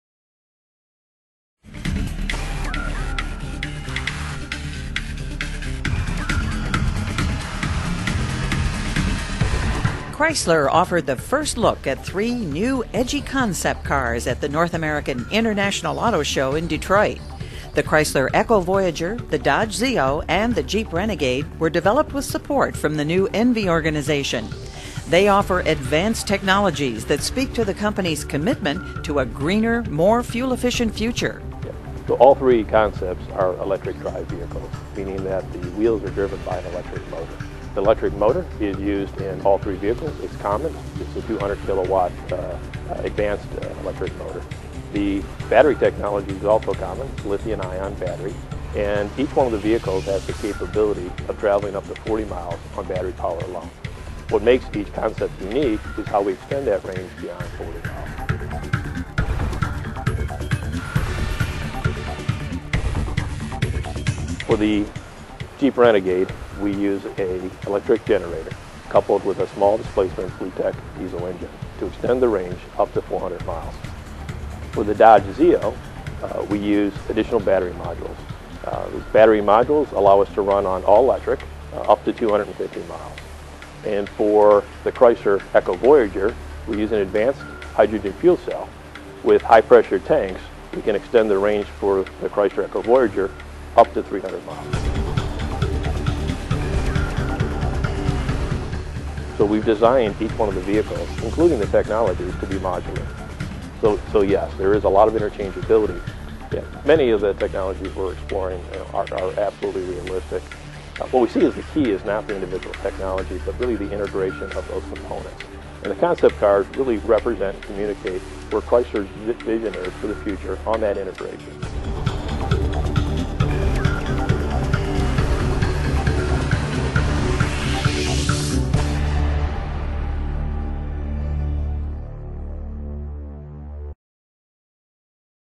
2008 NAIAS Chrysler Concepts - Lou Rhodes Interview
Chrysler LLC proudly presents three innovative concepts. The Jeep Renegade, the Dodge ZEO, and the Chrysler EcoVoyager. In this interview, Lou Rhodes of Chrysler's innovative ENVI organization, describes the green concepts to be displayed at the 2008 NAIAS.